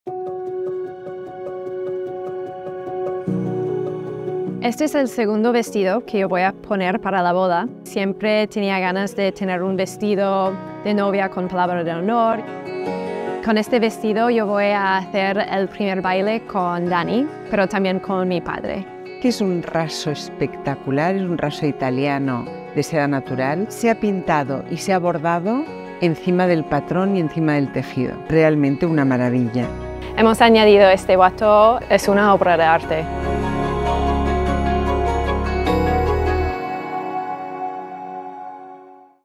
Anne-Marie Colling's Second Wedding Dress | Chapter 2 | Rosa Clará
Anne-Marie's second bridal look for her emotional first dances was a spectacular dress in a classic silhouette, with a strapless neckline and voluminous tulip skirt.

The gown was crafted in double “duchesse”, naturally dyed, silk that has been hand-painted in oil, using the spatula technique by an artist in Northern Italy, with the exclusive, jasmine-inspired design of romantic five-petal flowers. This was topped off with a subtle touch of gold dust for an authentic contemporary couture look that will be etched in the memory of all those present. The bride's second dress was completed with a tulle “Watteau” in the same design and technique as the dress.

“I feel very lucky to have been able to work with Rosa and her team, the whole process has been so exciting! I have always identified myself with the brand and I am extremely happy I get to wear four haute couture dresses designed for four very special moments of the wedding”, admits Anne-Marie.

El segundo look de @annemariec para sus emotivos primeros bailes fue un espectacular vestido de silueta clásica, con escote palabra de honor y una voluminosa falda tulipán.

Confeccionado en un doble raso duquesa de seda, se ha pintado a mano al óleo, con la técnica del espatulado por un artista del norte de Italia con un diseño exclusivo de románticas flores de cinco pétalos, inspiradas en el jazmín, y rematado con un sutil toque de polvo de oro para una auténtica imagen de alta costura contemporánea que quedará grabada en la memoria de todos los presentes. El segundo vestido de la novia se ha completado con un wateau de tul con el mismo diseño y técnica del vestido.

“Me siento muy afortunada de haber podido trabajar con Rosa y su equipo, todo el proceso ha sido muy emocionante. Siempre me he identificado con la marca y me siento increíblemente feliz de poder llevar cuatro vestidos de alta costura pensados para cuatro momentos muy especiales de la boda”, reconoce Anne-Marie.